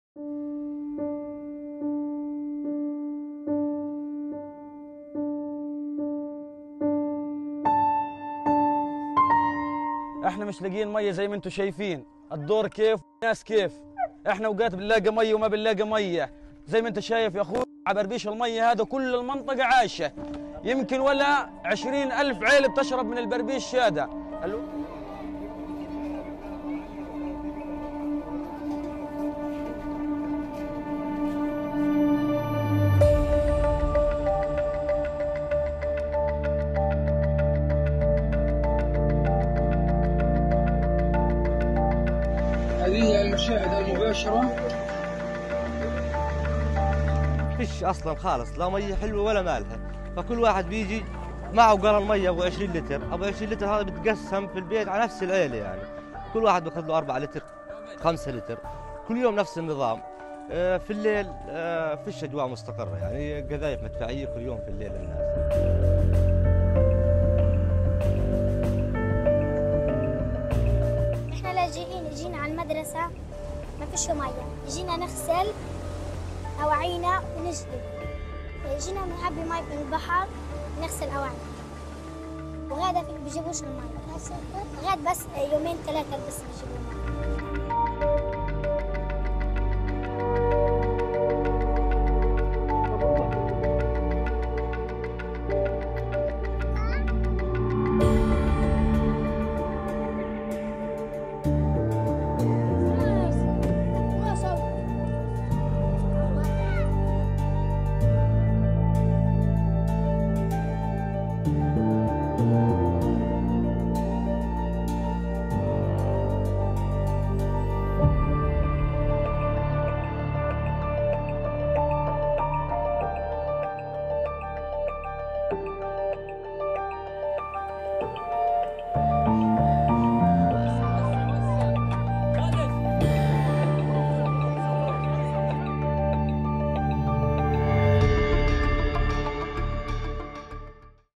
Gaza pati bez vode zbog izraelske opsade
Izraelska opsada Gaze stvorila je vodnu krizu. Stanovnici su prisiljeni satima čekati na oskudnu zalihu, dok su drugi primorani kupati se u zagađenoj morskoj vodi.  Zvaničnici UN-a kažu da se voda koristi kao ratno oružje.